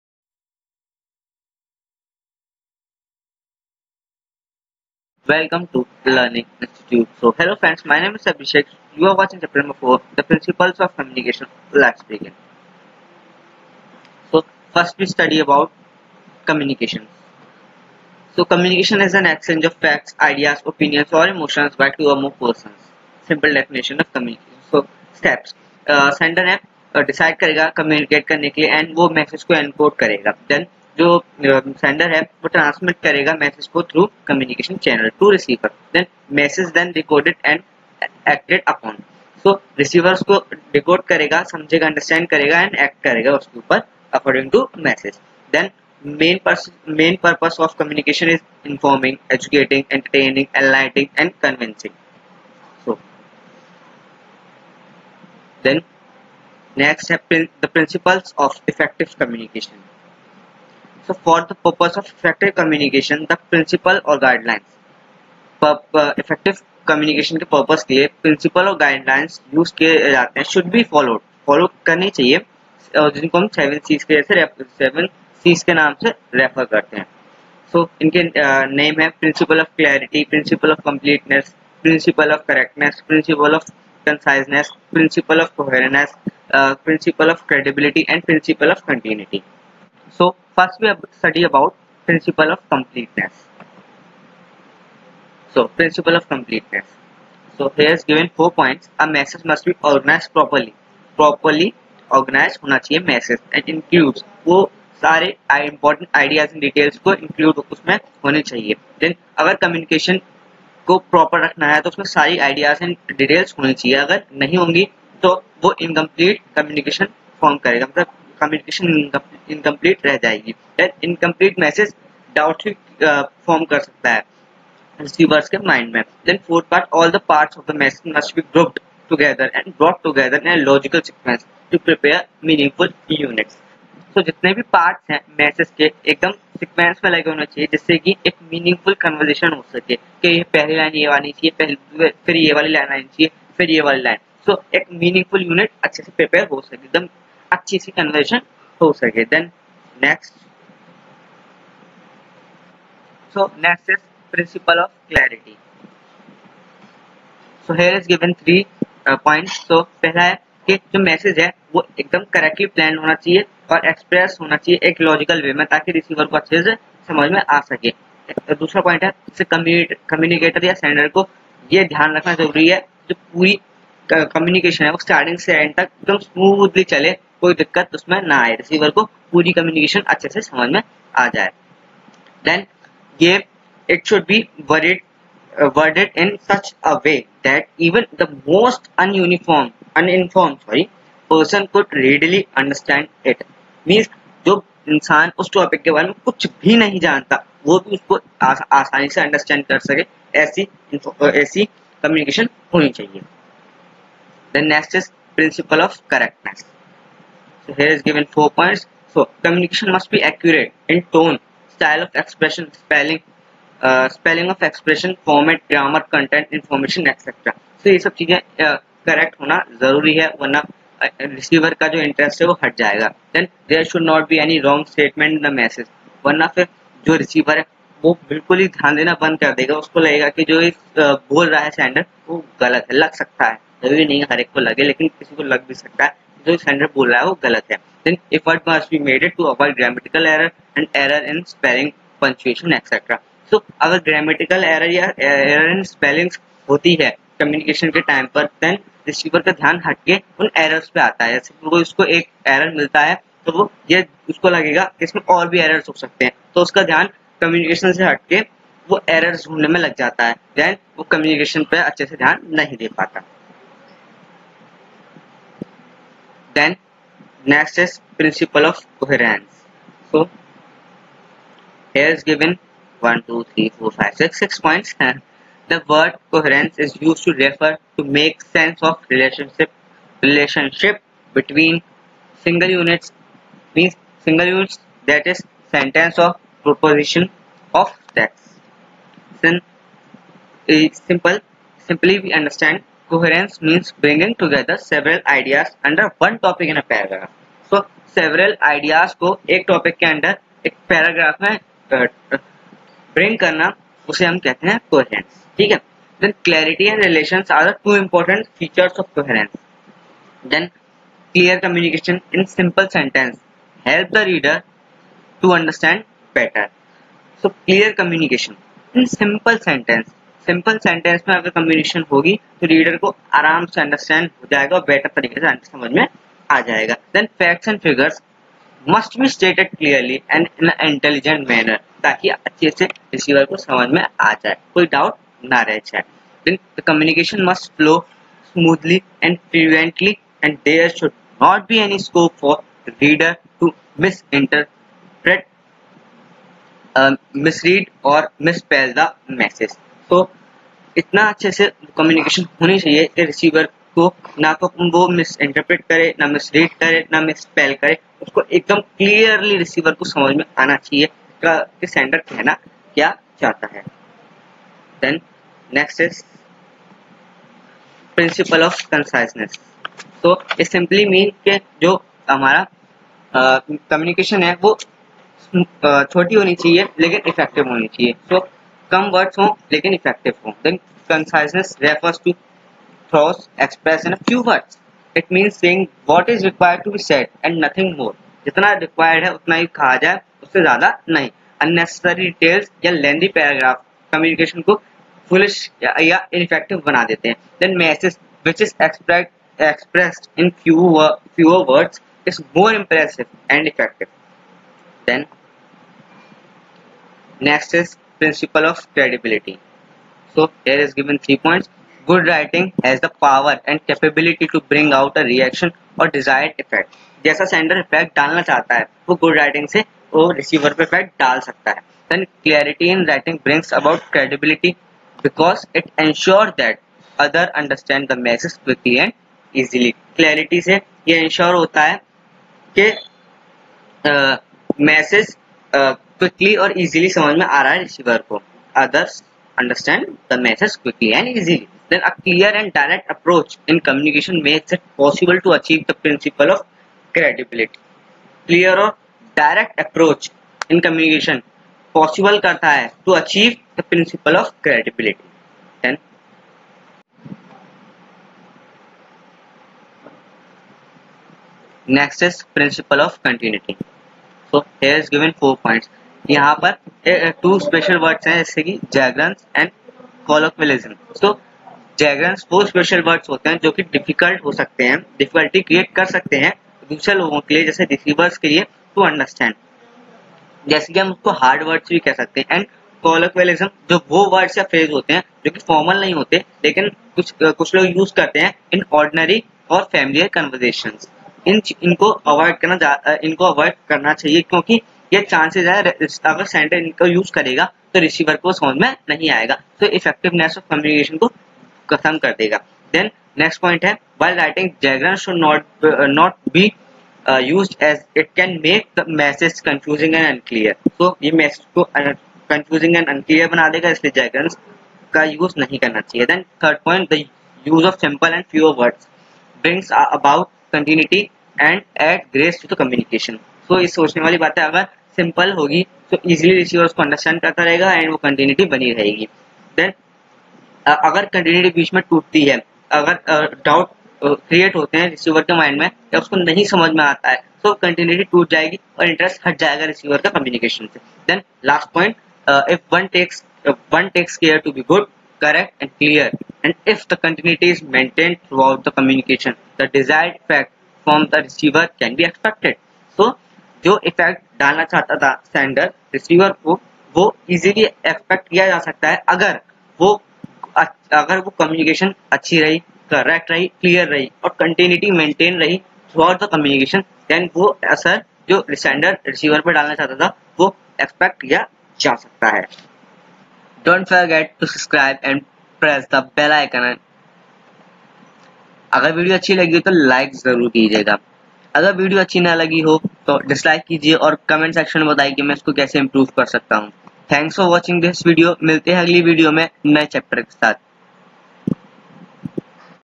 Class -- 10 || Chapter -- 4 -- The Principles Of Communication || Computer (code -- 402) IT
The 4th chapter of Information Technology ( Code -- 402 ) Computer is fully explained in this video.